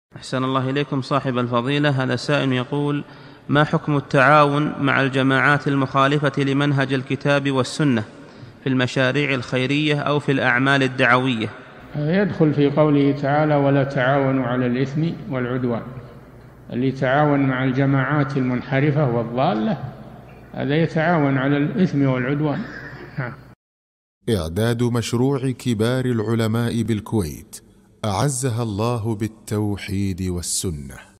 [306 -560] ما حكم التعاون مع الجماعات الضالة في المشاريع الخيرية؟ - الشيخ صالح الفوزان
-"السؤال: ما حكم التعاون مع الجماعات المخالفة لمنهج الكتاب والسنة في المشاريع الخيرية أو في الأعمال الدعوية؟
الجواب: يدخل في قوله تعالى {وَلَا تَعَاوَنُوا عَلَى الْإِثْمِ وَالْعُدْوَانِ} [المائدة: 2]، فالذي يتعاون مع الجماعات المنحرفة والضالة هذا يتعاون على الإثم والعدوان.
للاشتراك في قناة الشيخ صالح الفوزان
سؤال وجواب من شرح السنة للبربهاري ( 560 ) مقطع

للاستفسار والإقتراحات والدعم
رئيس الفريق
لمشروع كبار العلماء